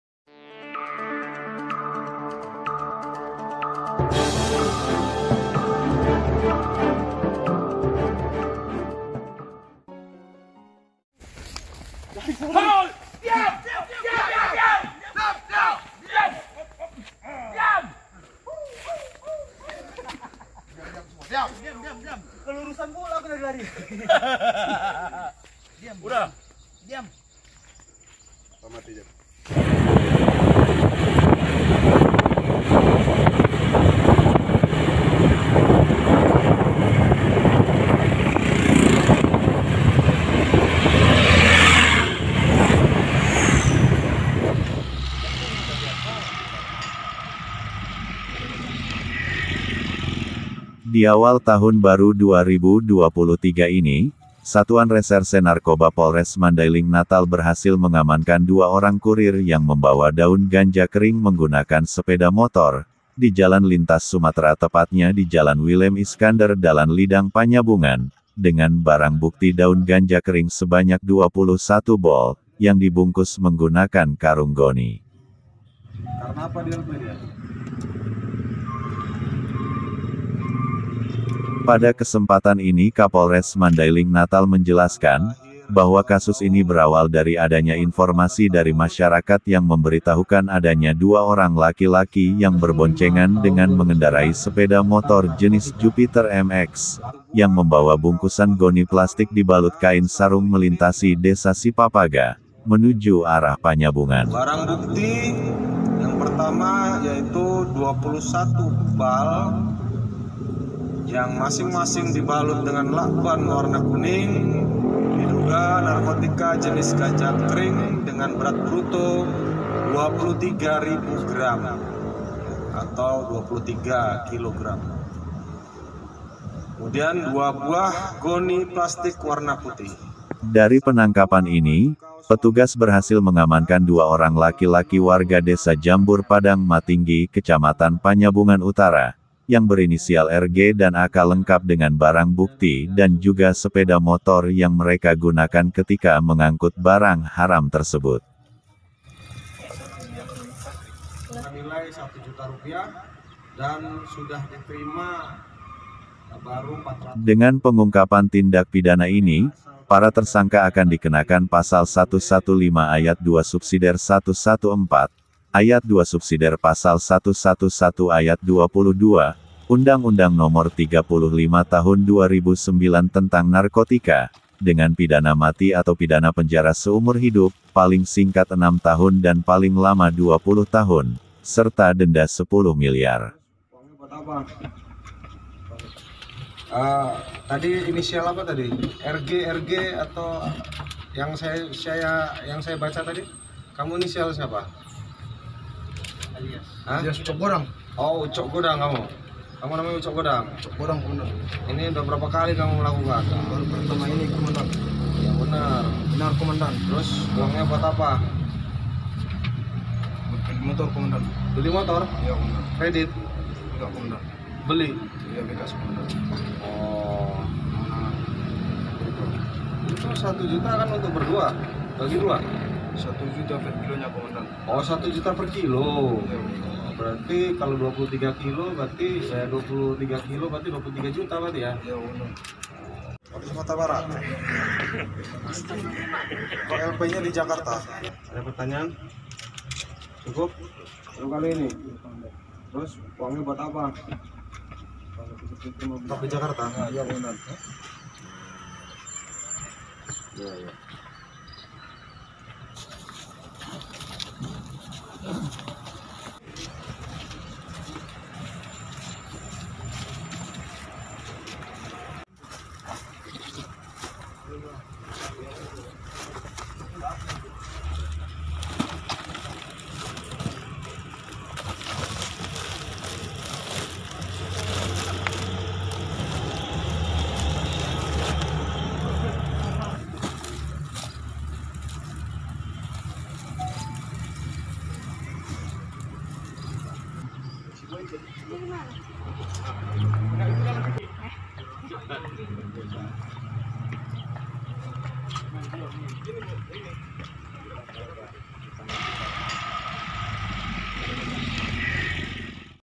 Gagal punya Motor dan Ponsel baru, Calo dan Ucok godang akhirnya menginap di Hotel Prodeo
Gagal punya Motor dan Ponsel Baru, inisial Calo dan Ucok godang Akhirnya menginap di hotel prodeo.

Kado di diawal tahun 2023 Polres Mandailing Natal

Kapolres Mandailing Natal AKBP H.M Reza Chairul A.S. S.IK, S.H., M.H. didampingi Kabag Ops Kompol M. Rusli, S.H, Kabag Ren Kompol A.R Siregar, S.H dan Kasat Narkoba AKP Irwan, S.H., M.H serta Kasubbag Dal Ops Ipda Hamdan S.H menggelar Press Release ungkap kasus penyalahgunaan narkotika di halaman mako Polres Mandailing Natal. Selasa, (03/01/2023).

Pada kesempatan ini, Kapolres Mandailing Natal menjelaskan bahwa kasus ini berawal dari adanya informasi masyarakat yang memberitahukan bahwa ada 2 orang laki-laki berboncengan  mengendarai Sepeda Motor Yamaha Jupiter MX dengan membawa bungkusan Goni Plastik dibalut Kain sarung dan Kaos melintasi Desa Sipapaga menuju arah Panyabungan. menindaklanjuti laporan masyarakat tersebut, Sat Narkoba Polres Mandailing Natal yang dipimpin AKP Irwan langsung melakukan pengintaian di depan Kantor Inspektorat Kel. Dalan Lidang pada tanggal 31 Desember 2022 sekira pukul 01.30 Wib Dini hari.

Dari pengintaian tersebut Personil Satnarkoba Polres Mandailing Natal berhasil mengamankan 2 Orang lelaki tersebut berinisial RG Alias Calo (18 Thn, Jambur Padang Matinggi) dan AK Alias Ucok Godang (21 Thn, Desa Jambur Padang Matinggi) dengan barang bukti 21 (Dua Puluh Satu) Ball yang dibalut dengan Lakban warna kuning dalam karung goni dan satu unit septor dengan nopol BB 2026 RM.

Kapolres Mandailing Natal AKBP H. M. Reza Chairul A.S S.IK., S.H., M.H mengatakan kedua tersangka yaitu RG Alias Calo dan AK Alias Ucok Godang bahwa barang haram tersebut yang dia dapat dari WI (dalam lidik) Warga Desa Simangambat Kecamatan Tambangan melalui OJI yang akan dikirim ke Jakarta melalui Jasa Transportasi dengan upah sebesar Rp. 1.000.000/Kg.

(Polres Mandailing Natal)